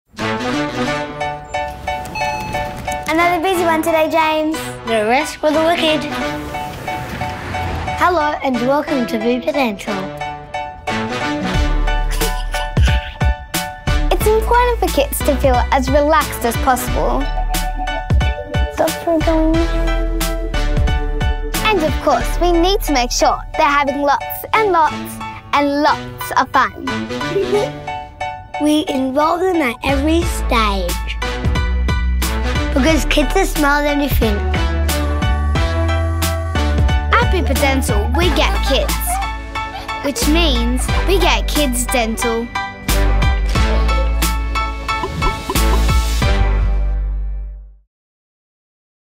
We get kids, which means we get kids dental
Who knew going to the dentist could be so much fun? Learn about a trip to the dentist and healthy habits at home from a kid’s perspective.